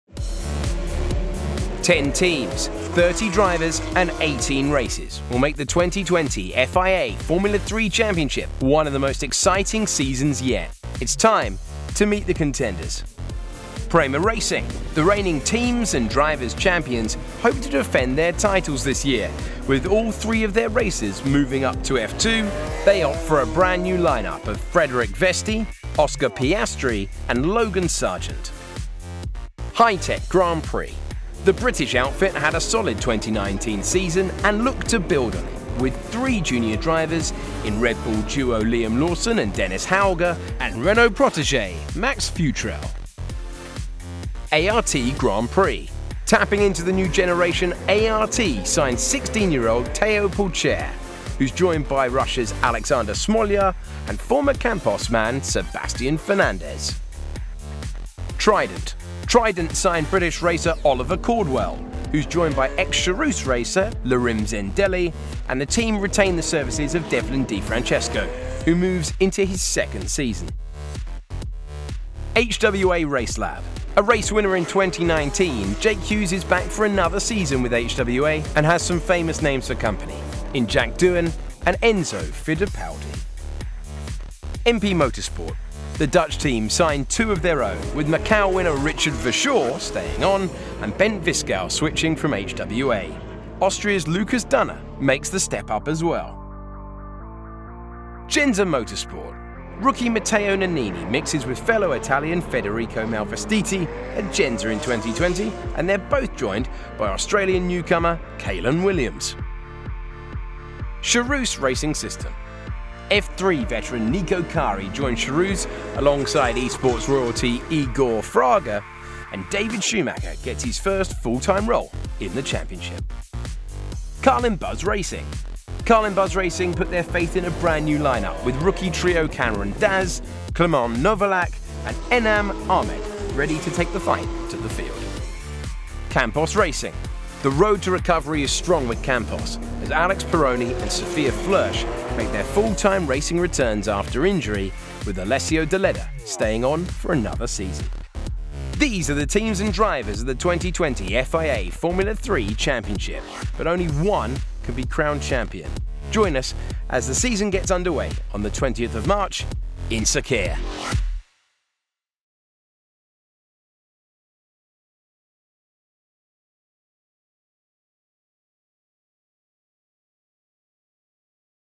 First Look at the 2020 FIA Formula 3 Championship
A huge grid full of immense talent. What's not to like?! Formula 3 testing in Bahrain gave us a first look at the teams and drivers of the 2020 season - let's take a look at them as we prepare to go racing in Bahrain once more.